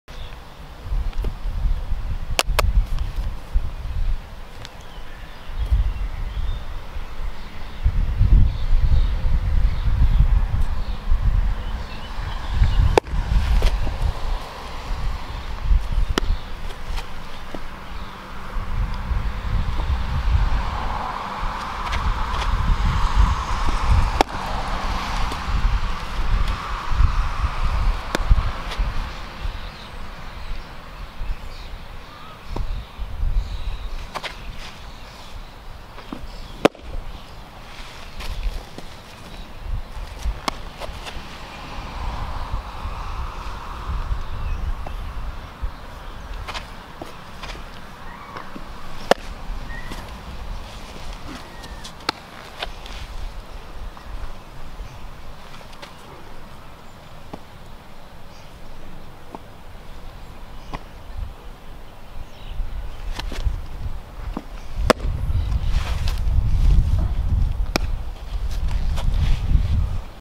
The SCOUTEE Smart Radar Gun - Baseball
Measure the speed of your pitch with SCOUTEE Smart Radar Gun.